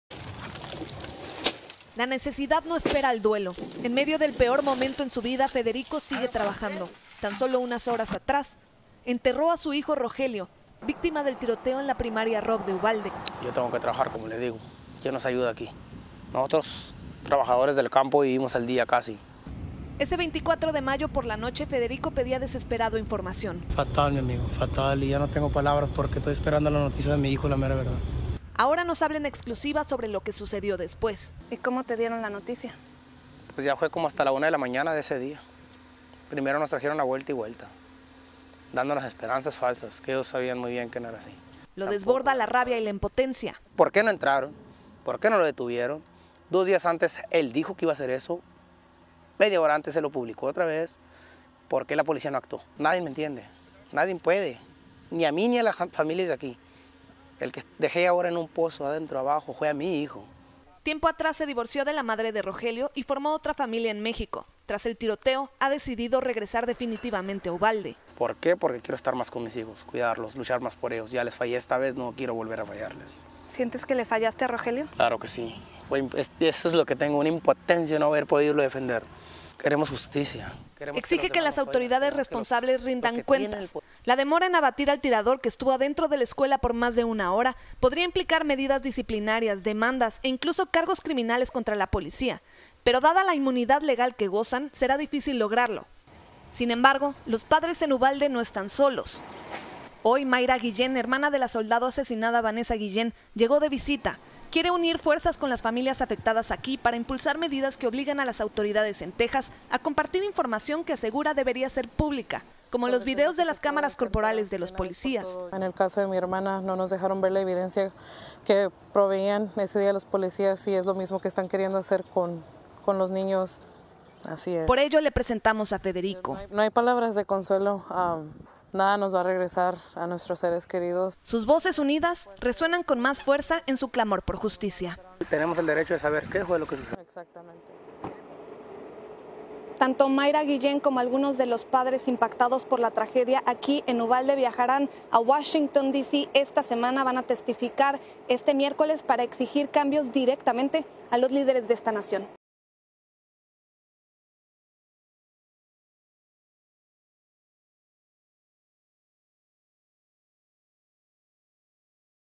El padre de una de las víctimas del tiroteo en Uvalde exige justicia | Noticias Telemundo
Video oficial de Noticias Telemundo. Federico Torres regresó al trabajo en el campo tras enterrar a su hijo Rojelio. Lo recuerda como un niño alegre que hablaba perfectamente el inglés y el español. No puede contener su dolor y su impotencia, le duele sobre todo no haber podido defenderlo.